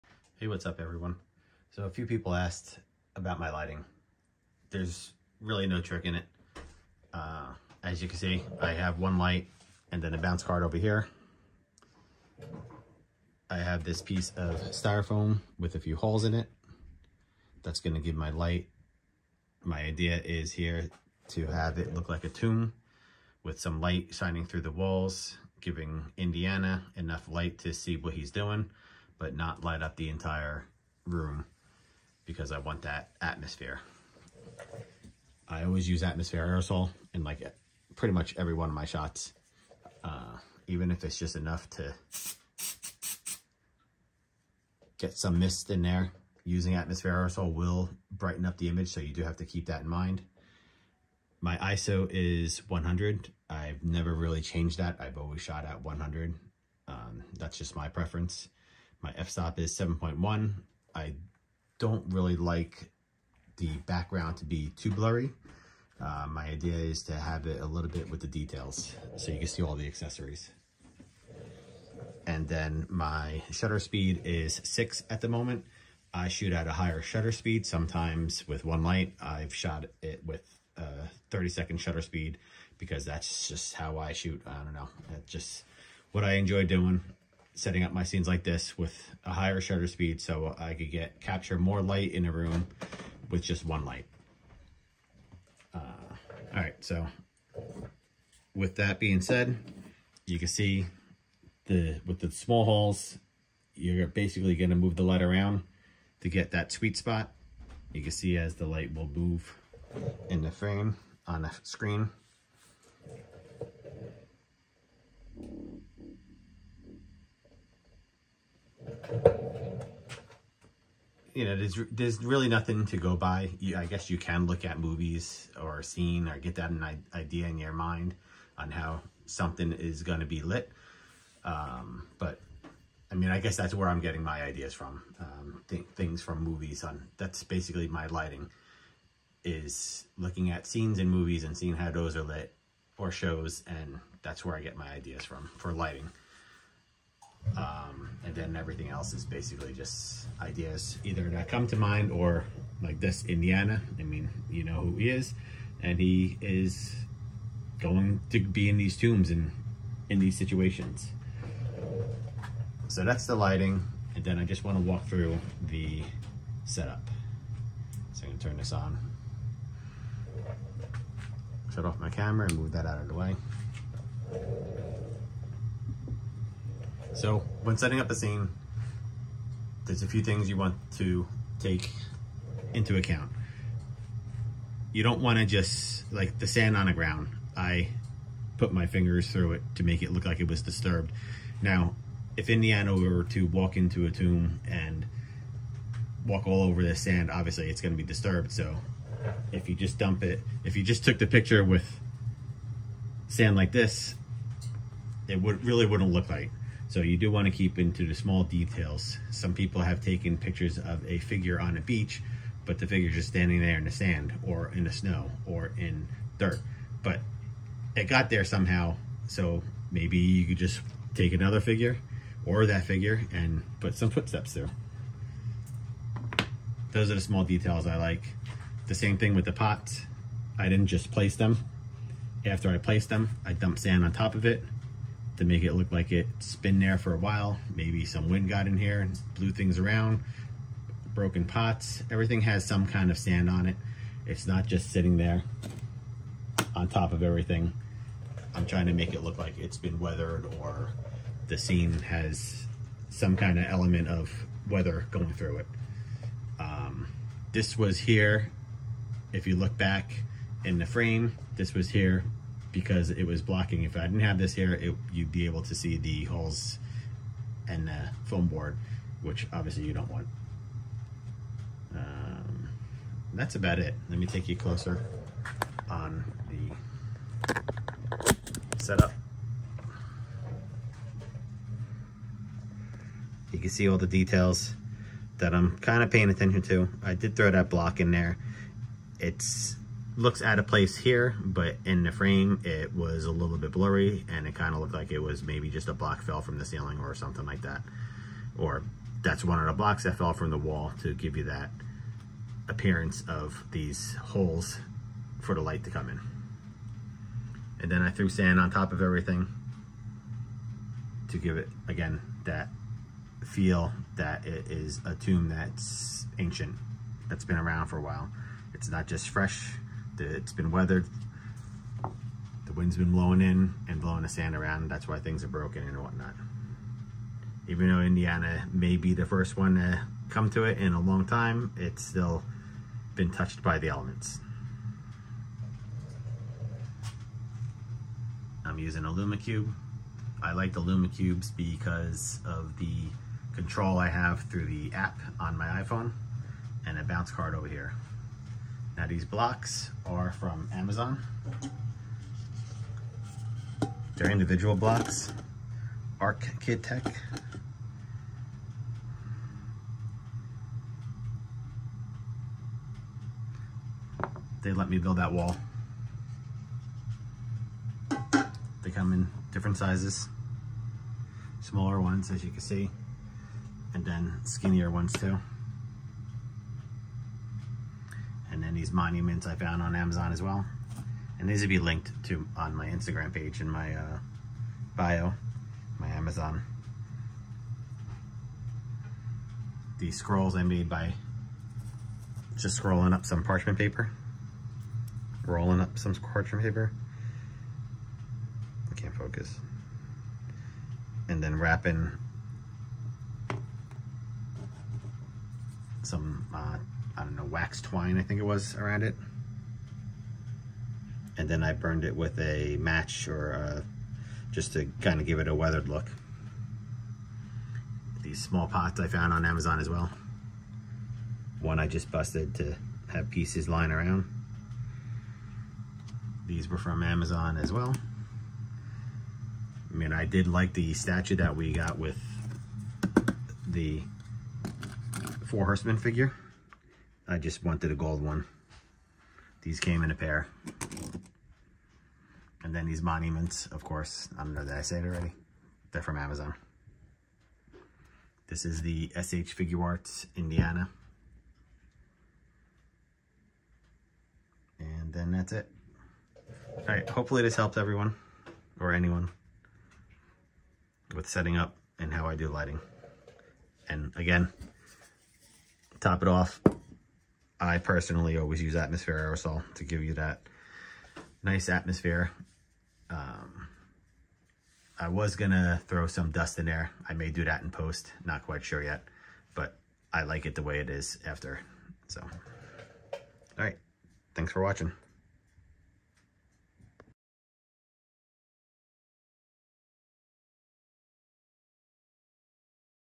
Toy Photography lighting for Egyptian tomb with Indiana
Quick review of lighting, camera settings and setup process of a photo for toyphotography